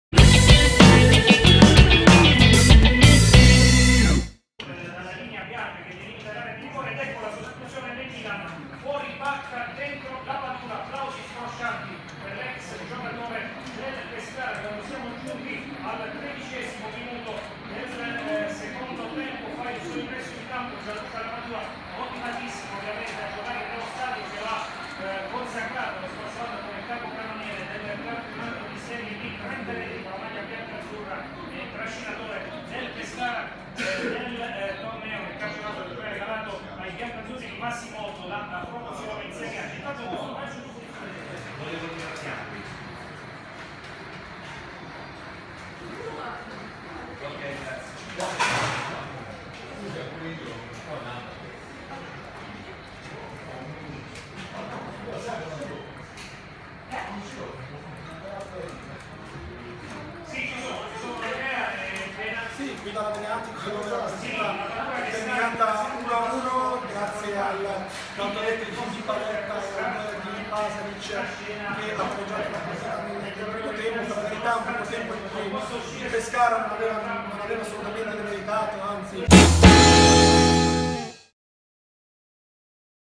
Lapadula torna a Pescara: l'ovazione dell'Adriatico e il giro di campo finale!
02/04/2017 Gianluca Lapadula torna a Pescara per la prima volta da ex: ovazione dello stadio Adriatico!